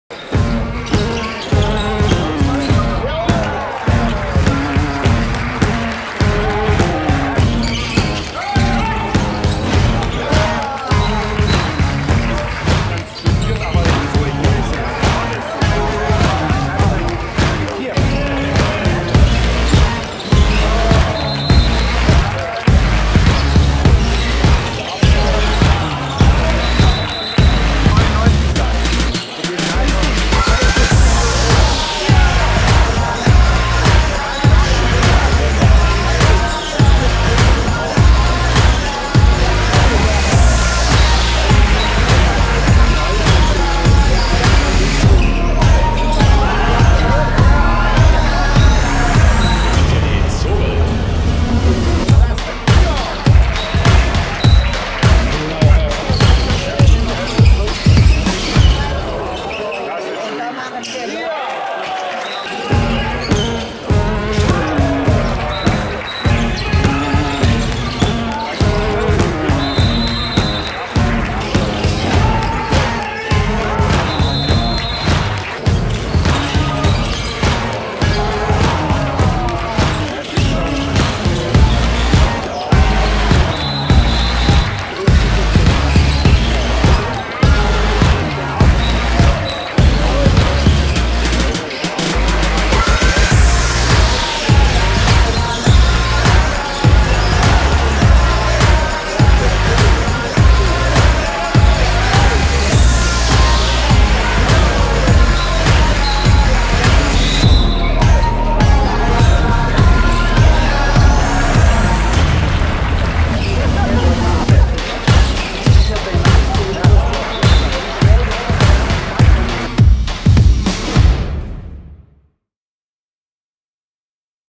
Tolle Tore, super Paraden: Highlights gegen Balingen! | HSG Konstanz
Tolle Tore, super Paraden: Die Highlights aus dem Test gegen Balingen! 🔝

Das zweite Testspiel gegen Erstligist Balingen-Weilstetten war schon über weite Strecken recht ansehnlich (26:33/13:14). Da waren schon einige schöne Aktionen dabei, die uns richtig viel Freude bereitet haben. Seht selbst!

Heute geht's um 18 Uhr in der Schänzle-Sporthalle weiter gegen den Schweizer Nationalliga-A-Club HC Kriens-Luzern, der in der letzten Saison im Playoff-Viertelfinale um die Meisterschaft stand.

🤽🌊💦 Gestern Abend holte sich unser Team zudem neue Eindrücke beim Playoff-Spiel des SC Kreuzlingen Wasserball um die Schweizer Meisterschaft. Wir bedanken uns sehr für die Einladung, gratulieren zur 2:1-Führung und wünschen noch viel Glück für die restliche Endrunde! 🤽🌊💦

Offizieller YouTube-Kanal der HSG Konstanz, 2. Handball-Bundesliga.